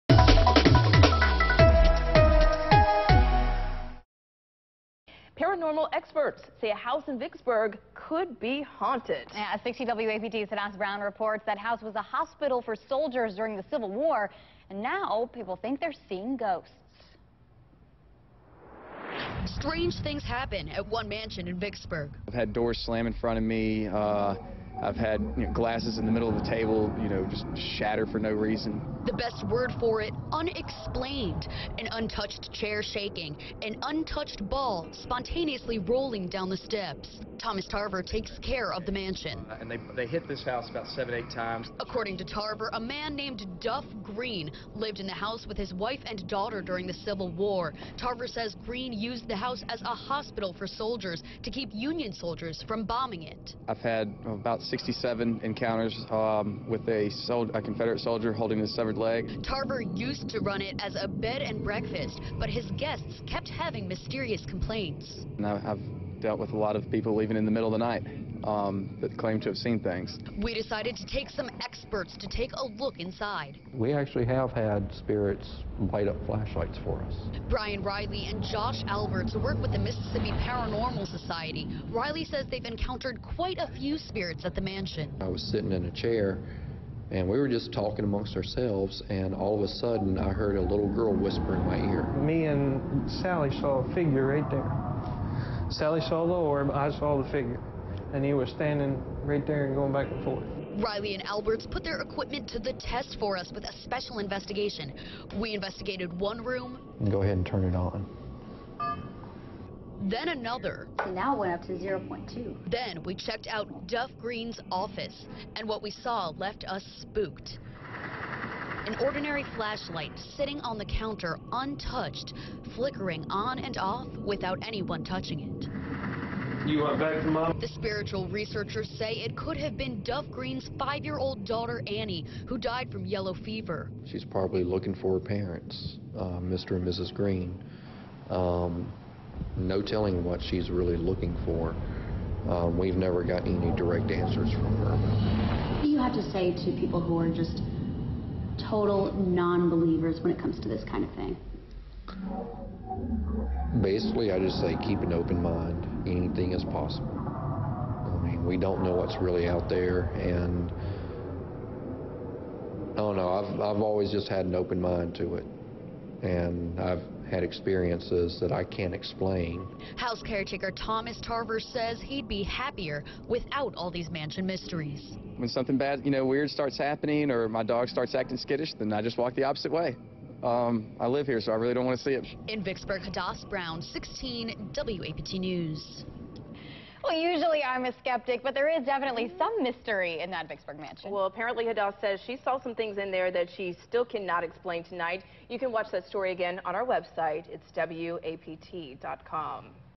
Special Report: Vicksburg Haunted House
Paranormal experts say a house in Vicksburg could be haunted.